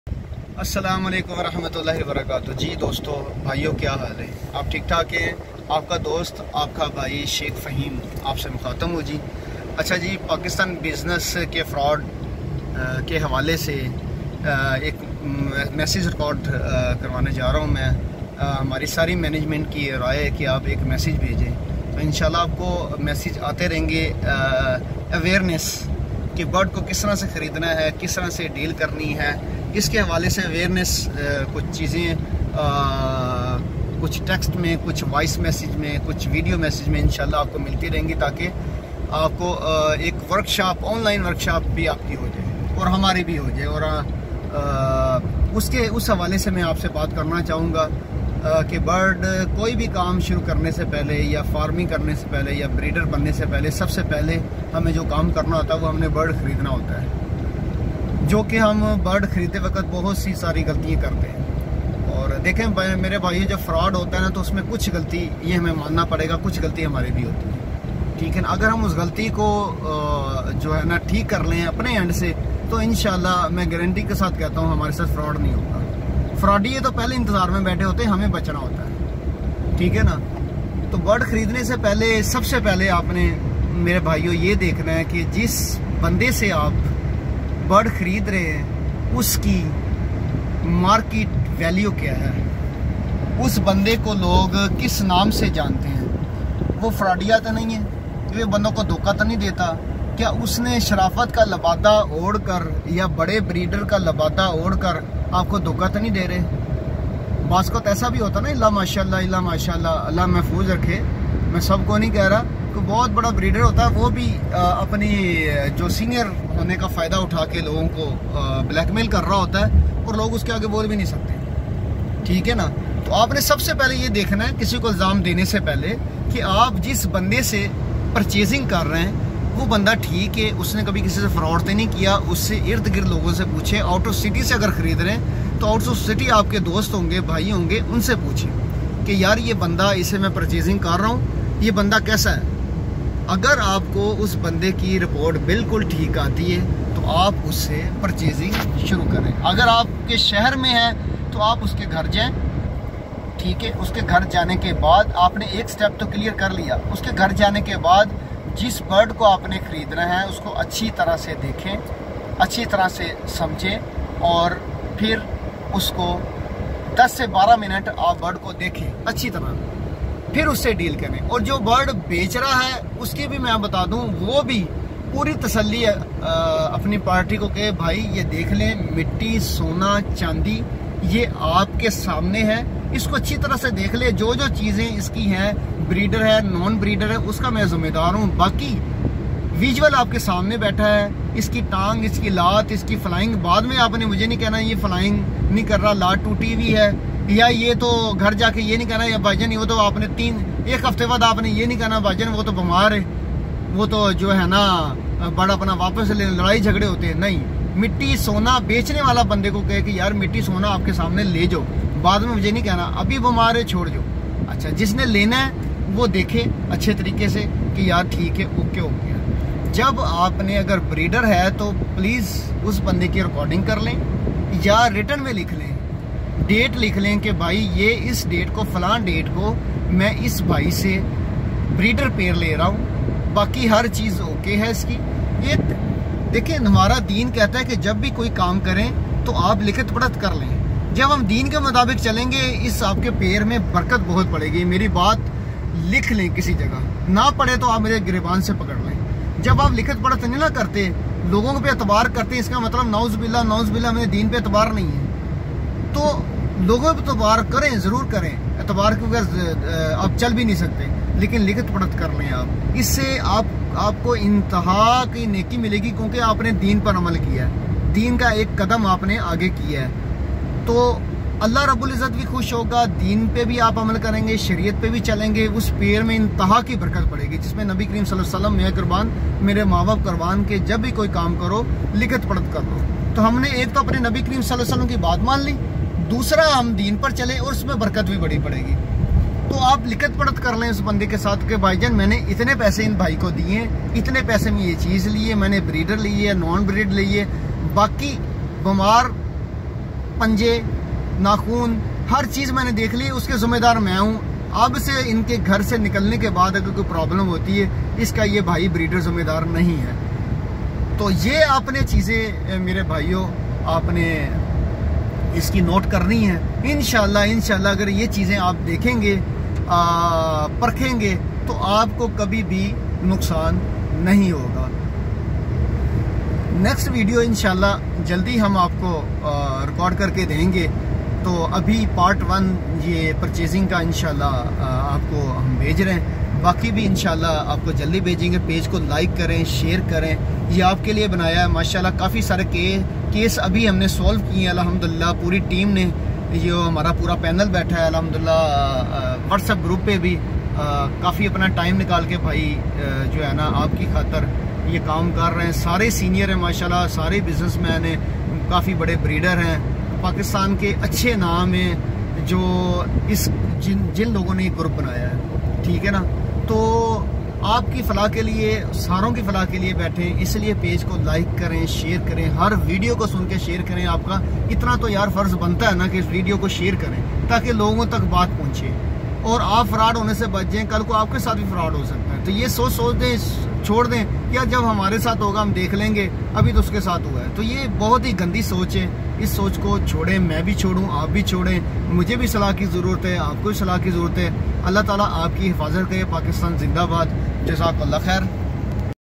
اگر خریداری کرنے سے پہلے ان باتوں کا خیال رکھیں
اگر خریداری کرنے سے پہلے ان باتوں کا خیال رکھیں گے تو 100% فراڈیوں  سے بچ جائیں گے ۔پہلا حصہ آن لائن ورکشاپ کا